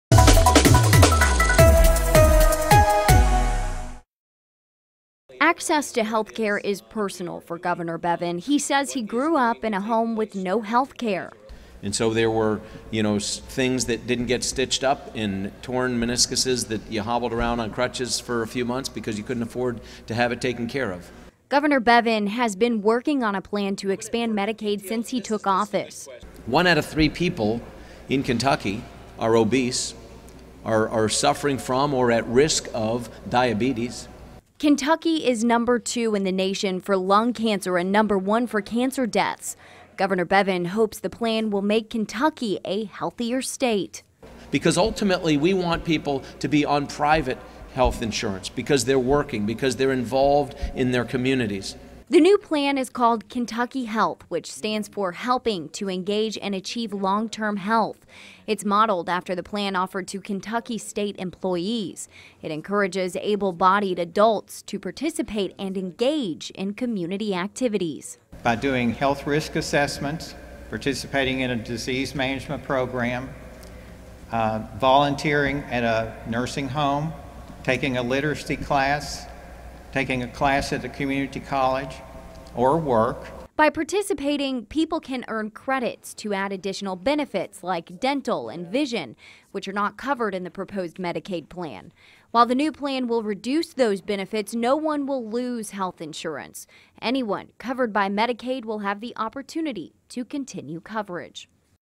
Kentucky Gov. Matt Bevin announces Medicaid transformation proposal
Kentucky Gov. Matt Bevin announced Wednesday his plan to transform Kentucky’s Medicaid program.